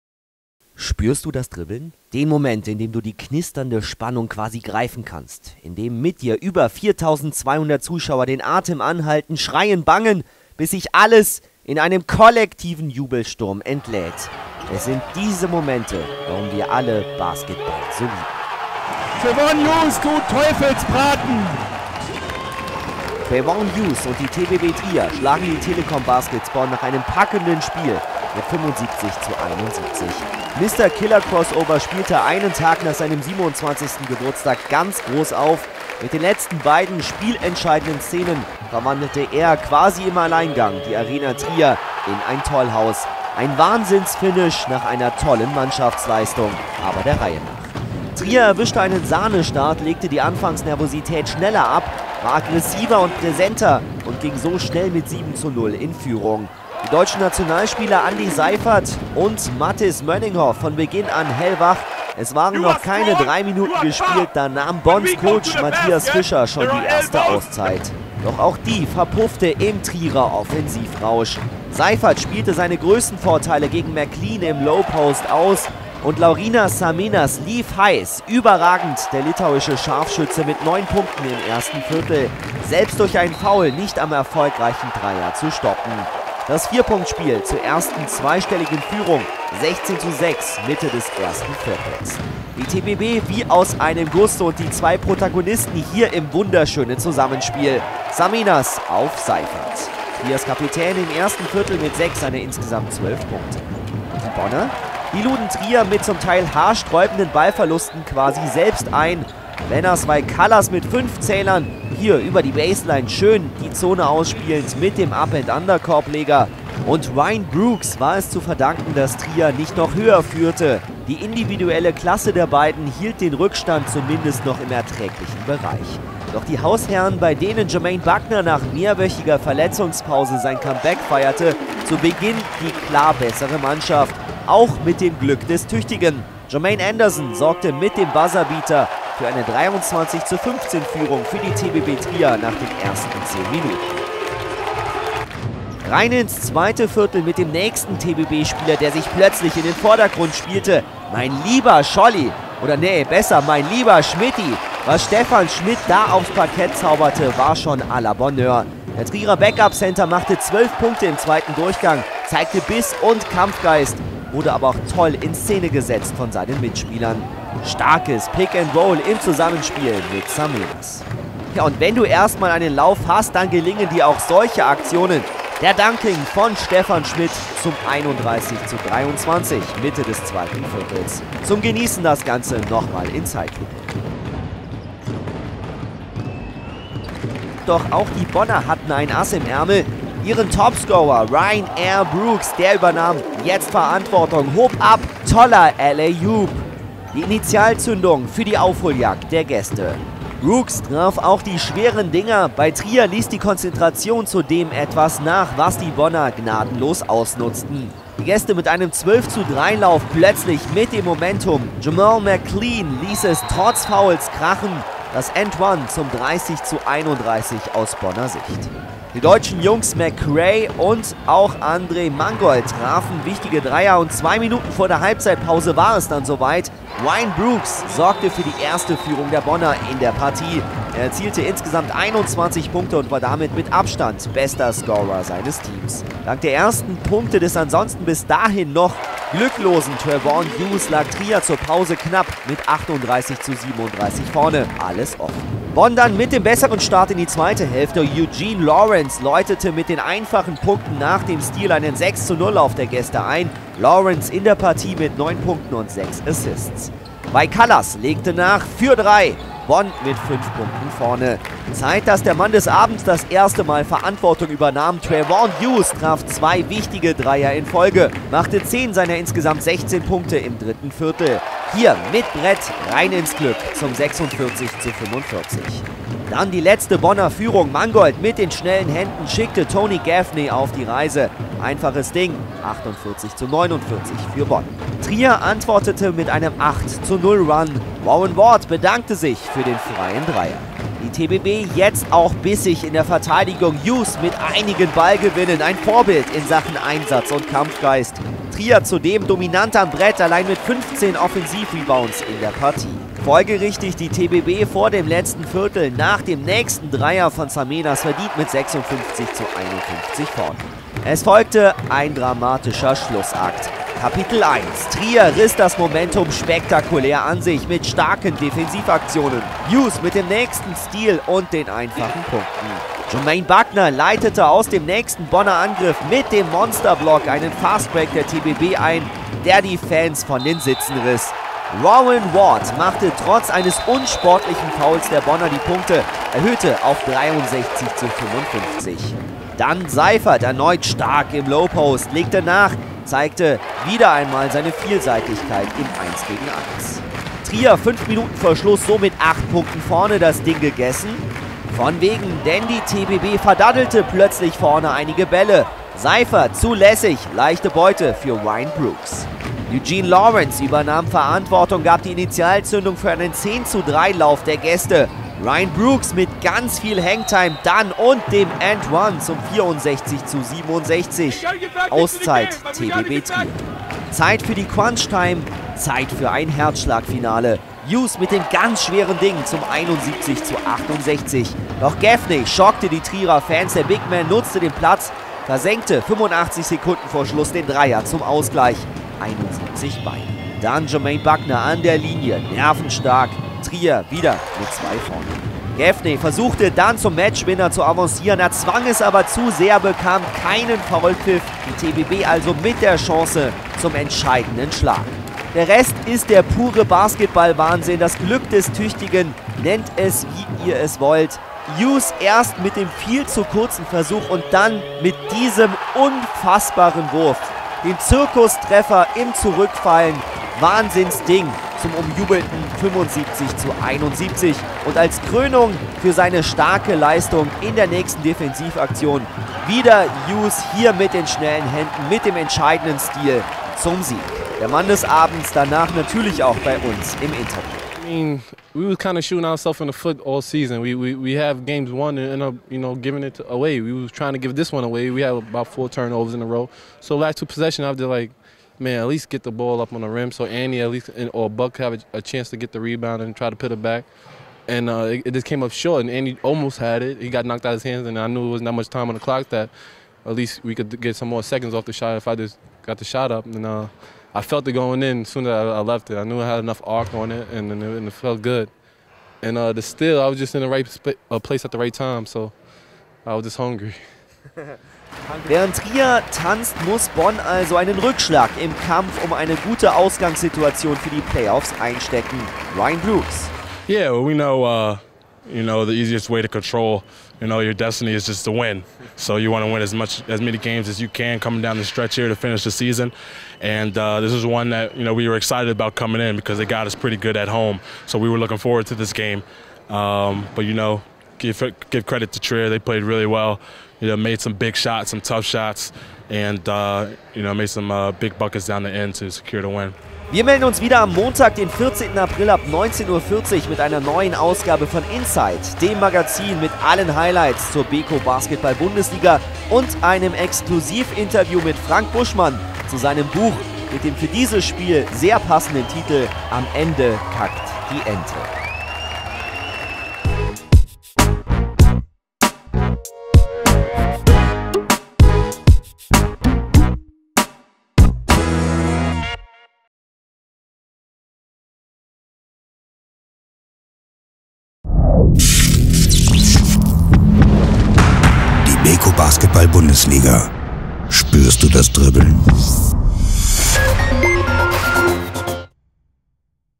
Hughes mit dem Hammer - Schmidt fliegt - Trier siegt
Es war ein hart umkämpftes, ein packendes Spiel mit Derbycharakter zwischen der TBB Trier und den Telekom Baskets Bonn, bei der die Entscheidung vor über 4200 Zuschauern erst in den letzten Sekunden fiel. Mann des Abends war der Trierer Topscorer Trevon Hughes mit 16 Punkten, 8 Asssist und 2 gescouteten Steals.

Spielbericht: basketball-stream.de
Redaktion, Kommentar: Chris Schmidt
Schnitt: David Vilter
Kamera: Marc Blasius, Ralph Arnold, Frank Schmitt